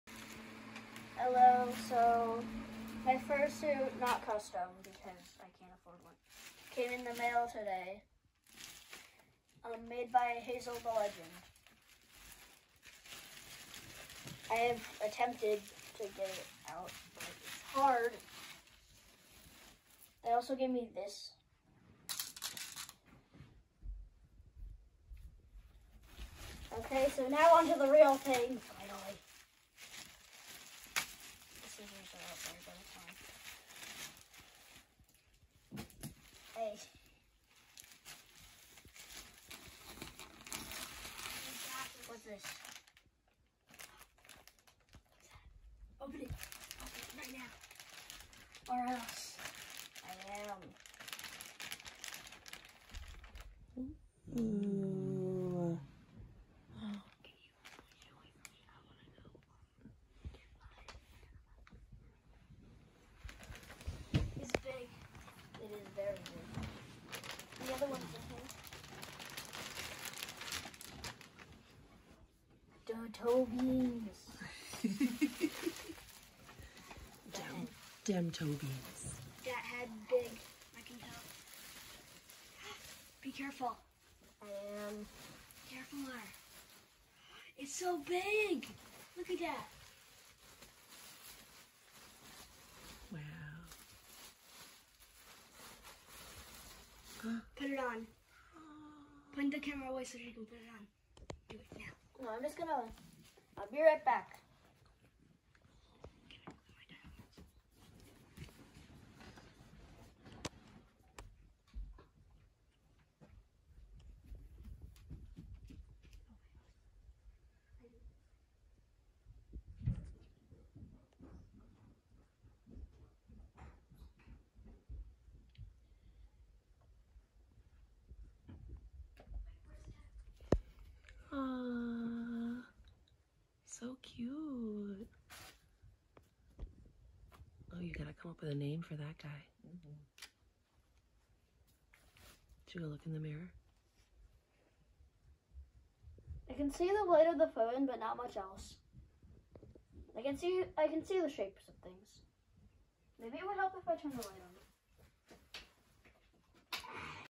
Thank you Hazel The Legend! (Read desc)
Hi Beans! (Ima call you that now XD) I got my first fursuit for my birthday from Hazel the Legend on etsy! Meet Cream! Cream goes by He/They. Bye lol